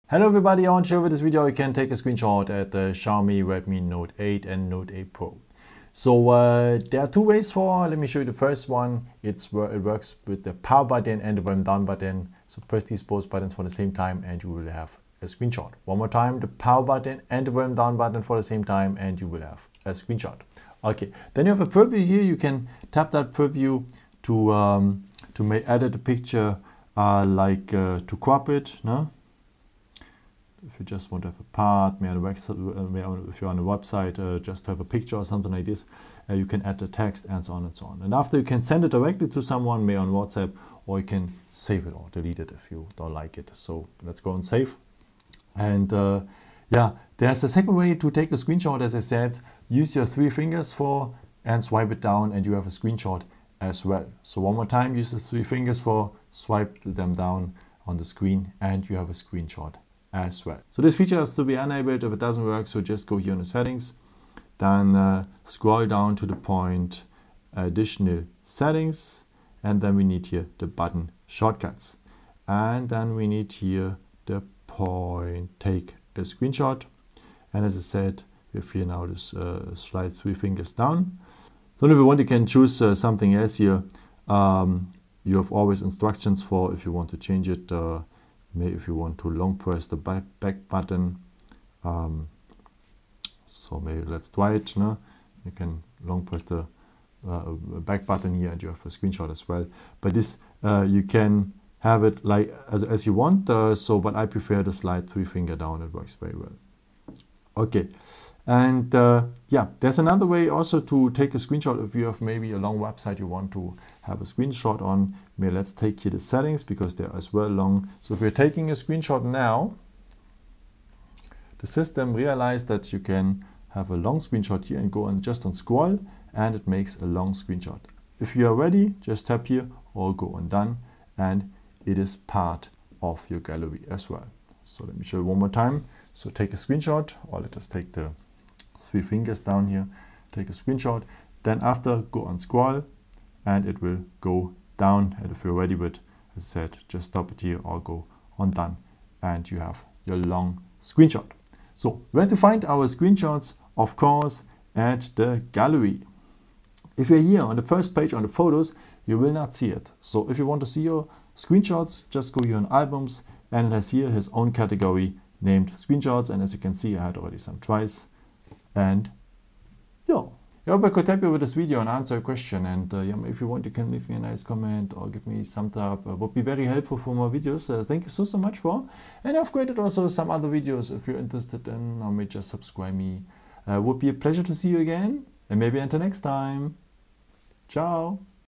Xiaomi Redmi Note 8 (Pro): How to take a screenshot/capture? (incl Scrollshot)
With this video, I want to show you, how you can take a screenshot/capture, with the Xiaomi Redmi Note 8 (Pro). I will show you also, how you can take a scrollshot (long screenshot).